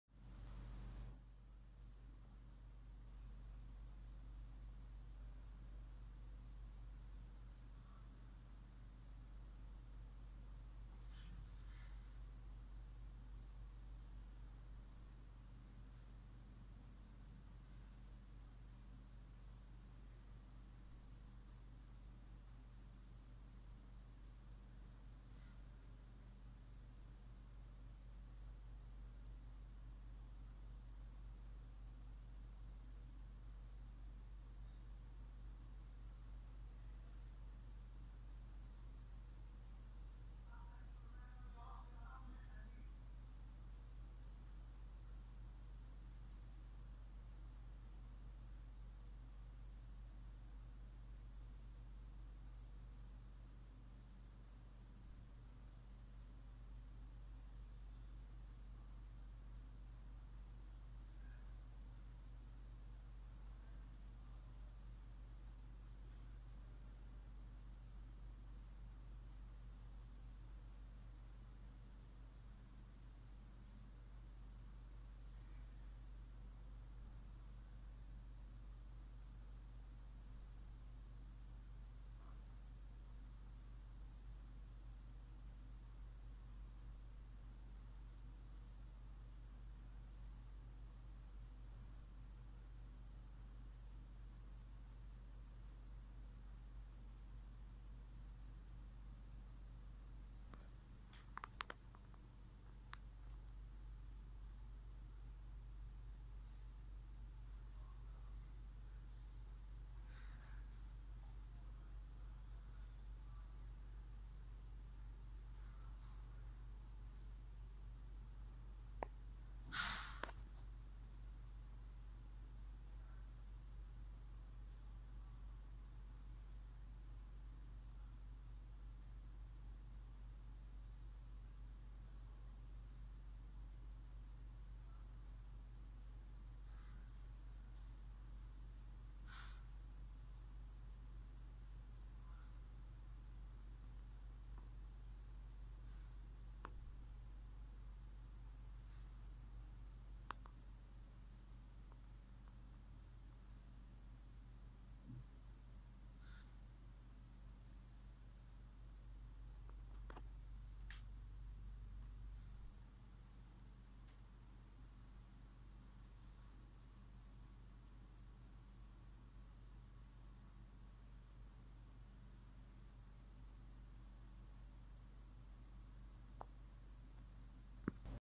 4/9/20 The neighbour moved.(Proof 2)
My Hyposthesis is that many people were entering his appartment from the window,from the street and they were trying to make me give things from the window of my toilet.